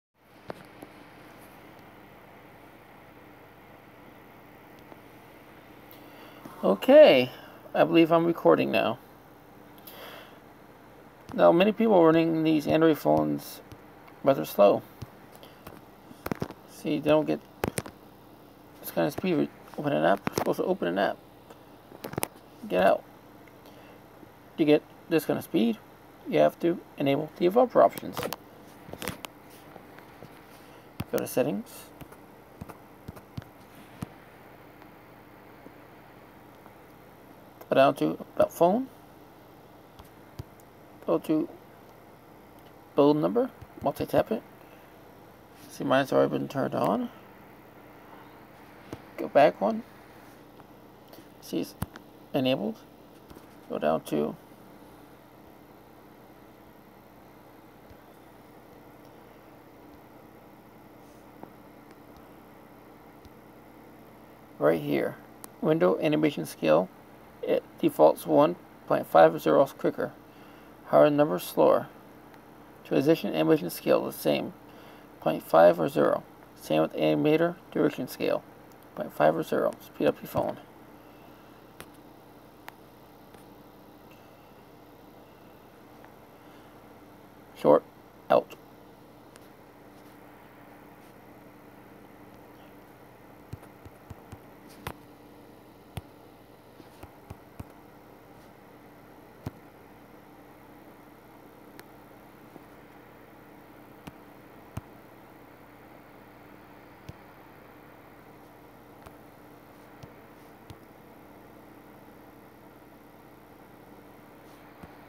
How to speed up your phone
As I was recording game play,  I thought I would share something with you all.  How to speed up your phone!